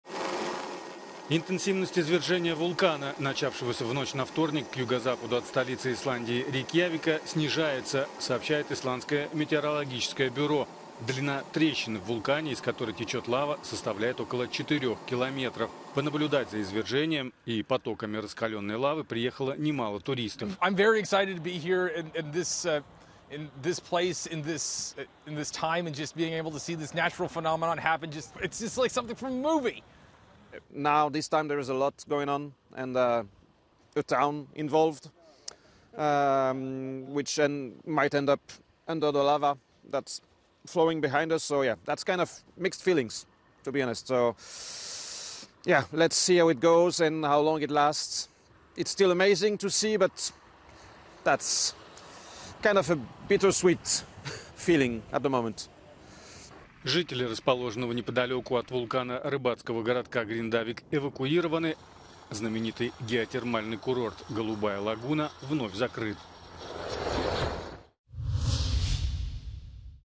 Интенсивность извержения вулкана на исландском полуострове Рейкьянес снизилась
Согласно обновленным данным Метеобюро Исландии, интенсивность извержения вулкана на полуострове Рейкьянес снизилась. Жители расположенного поблизости города Гриндавика эвакуированы, однако это природное явление привлекло на полуостров немало туристов.